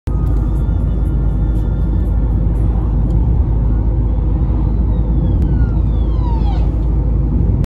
North West Ambulance Service DCA Responding
Seen here is A027 a Double Crewed Ambulance Responding on 09/11/22 Near Cockermouth.